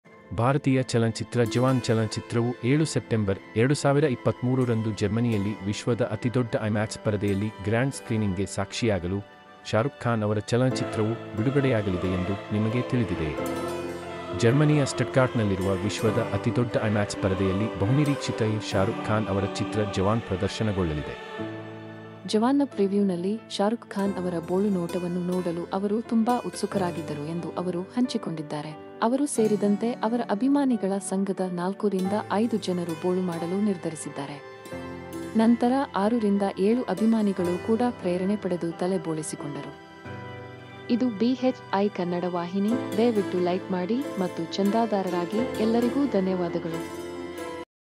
Shah Rukh Khan’s Film Jawan To Witness Grand Screening At World's Largest IMAX Screen In Germany
Smart Knowledge, Smart Mind, has no Holiday.